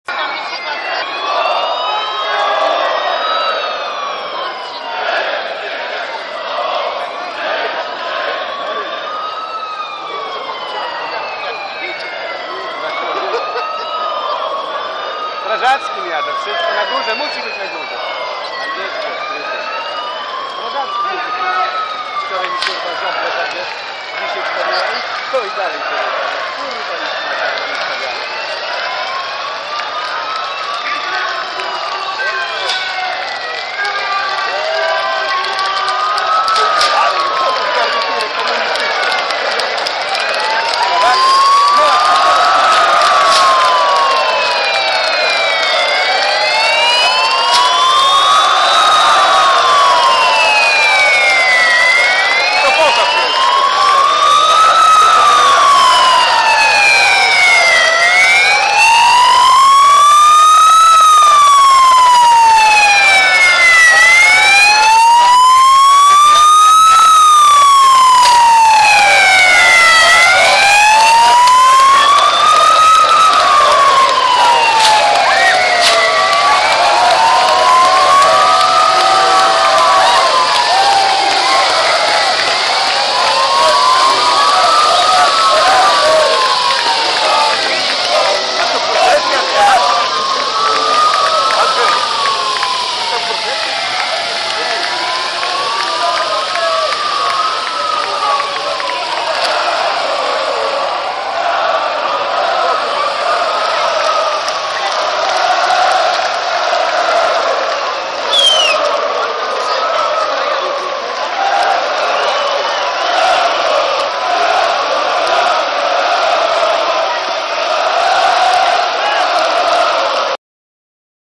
Jagiellonia Białystok - Puchar Polski (Feta)
Feta w centrum Białegostoku z okazji zdobycia przez Jagiellonię Pucharu Polski. Przejazd i przywitanie zawodników oraz sztabu szkoleniowego.

Białystok, Plac Uniwersytecki, 24.maja 2010.